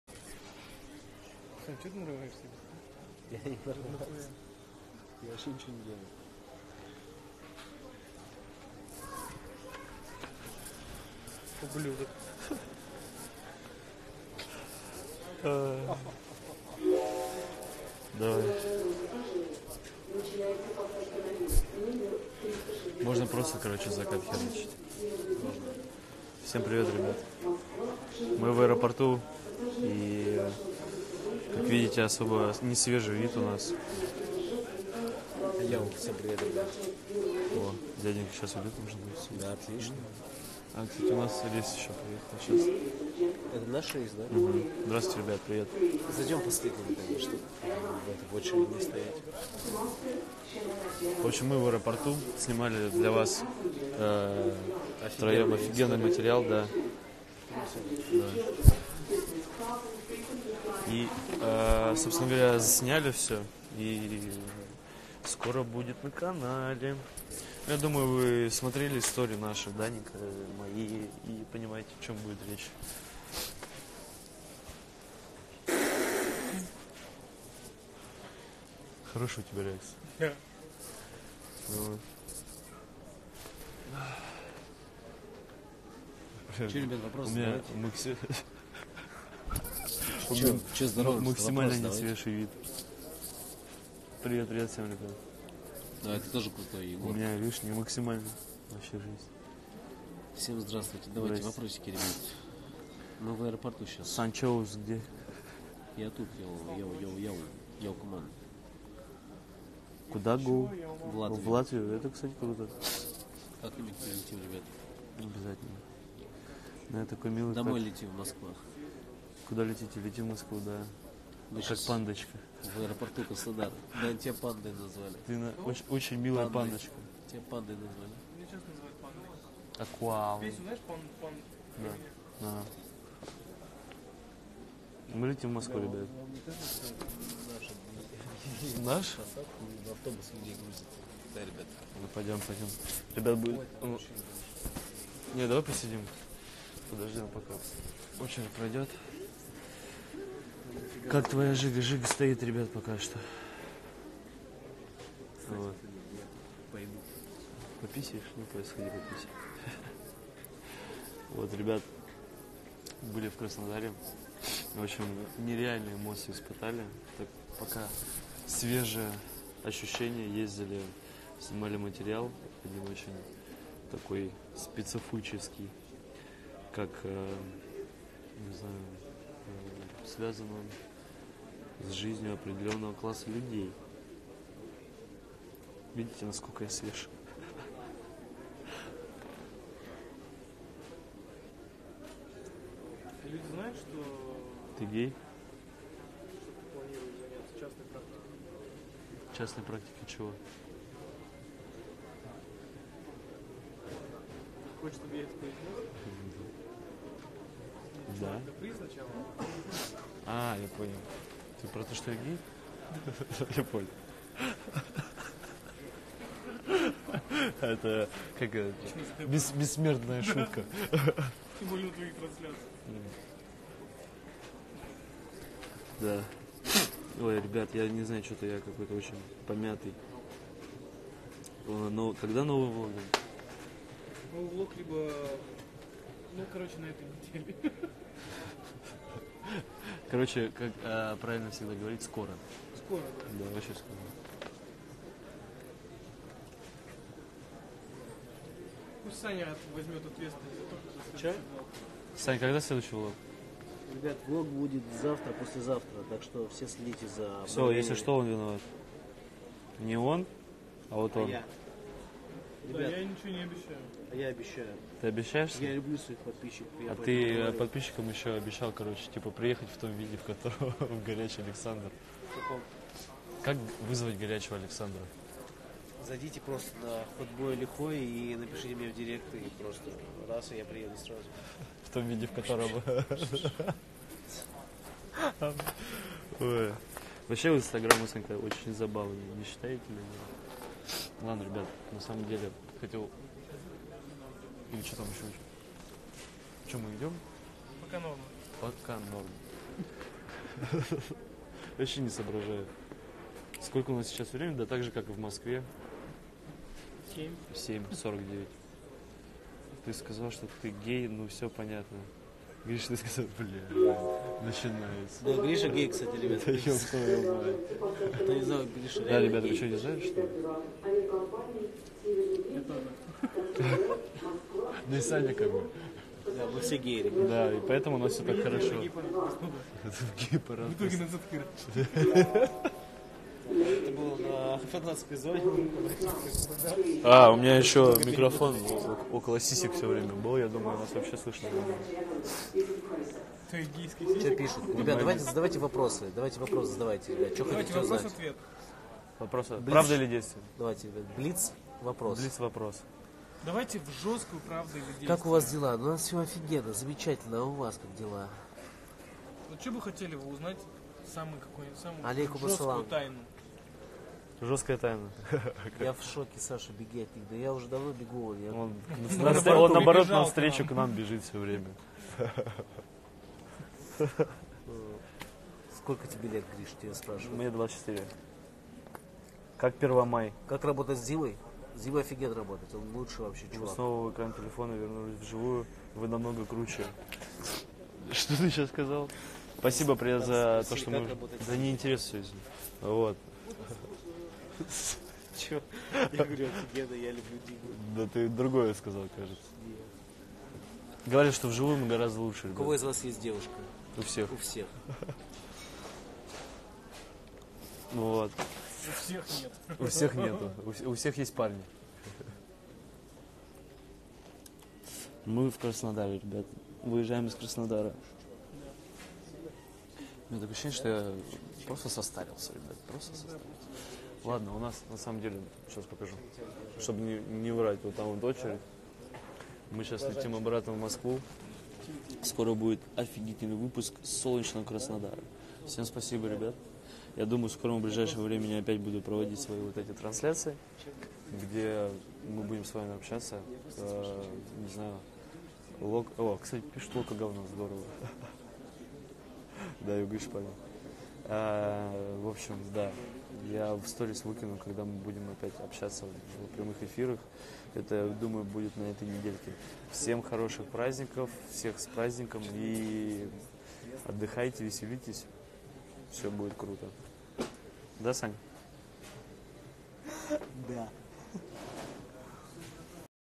Трансляция Григория Давыденко в инстаграме (01.05.18)
Прямой эфир в инстаграме Григория Давыденко, друга Димы Масленникова.